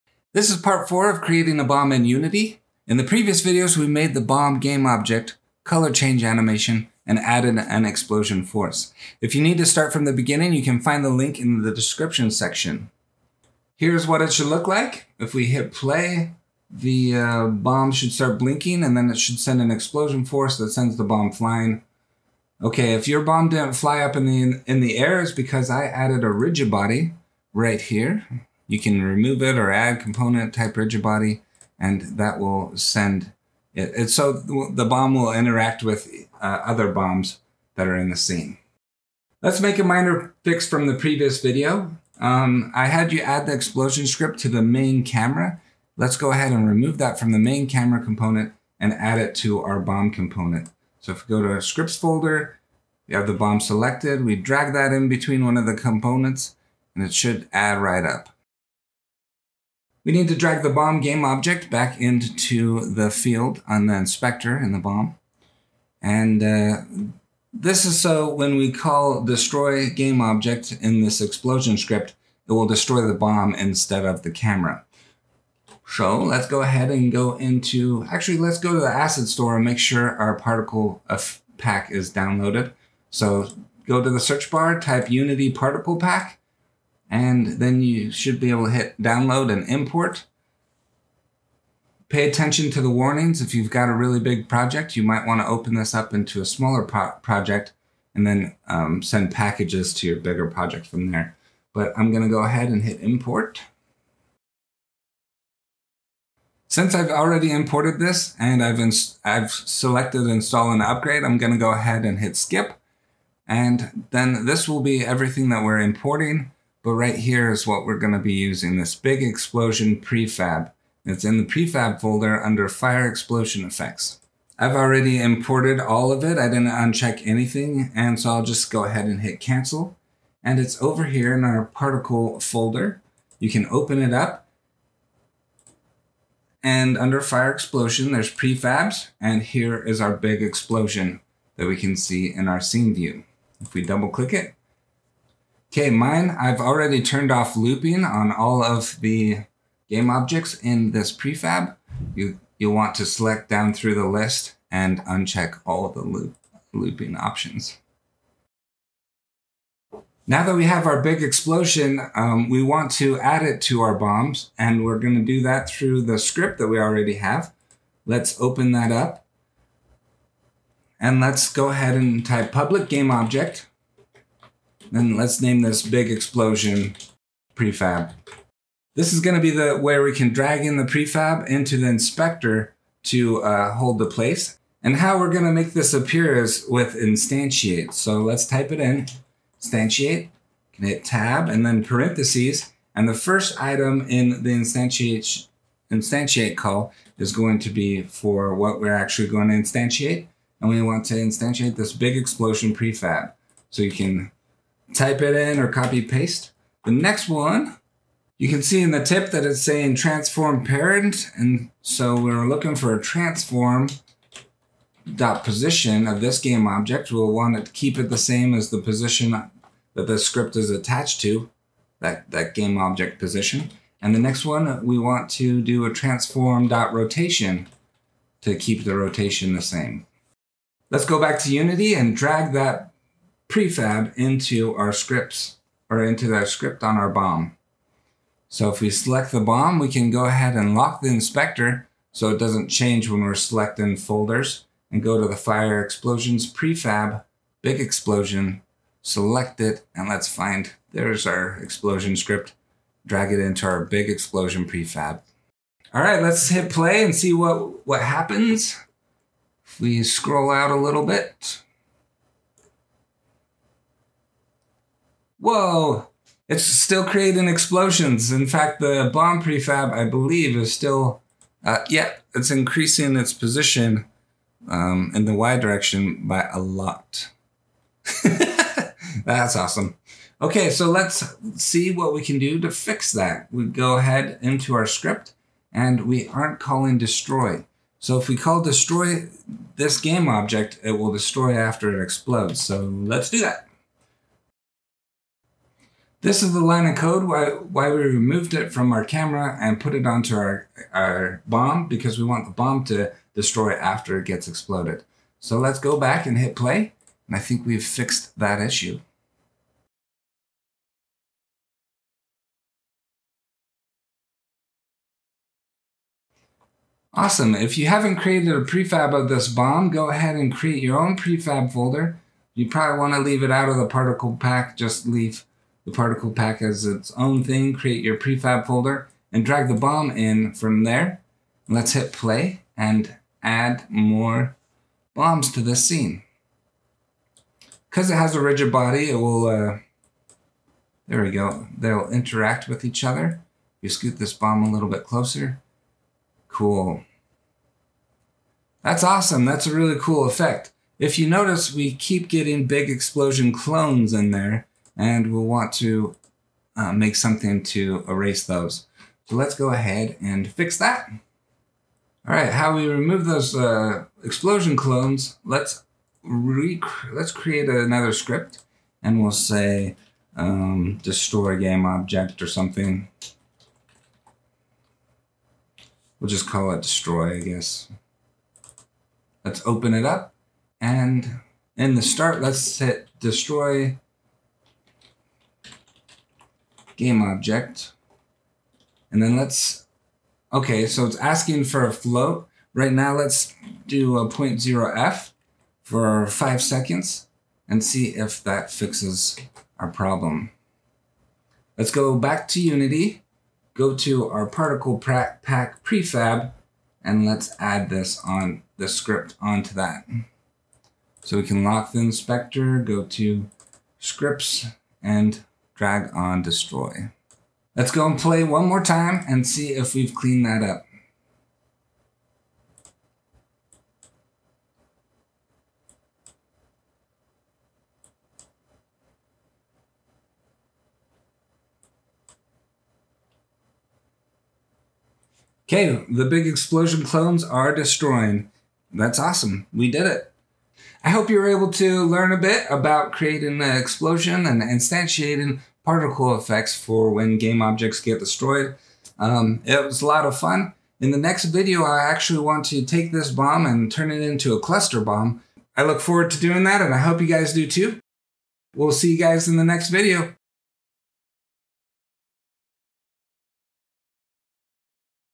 Creating A Bomb In Unity #4- Adding A Particle Effect - Instantiating
Creating A Bomb In Unity to put into our own games!! We are going to learn about instantiating a game object that has a premade particle effect. This will allow us to have an added particle effect to our bomb.

Let's learn how to make a Bomb In Unity. In this video, we are adding more code to our script and it will instantiate our explosions particle effect. This will be lots of fun and hopefully very informative.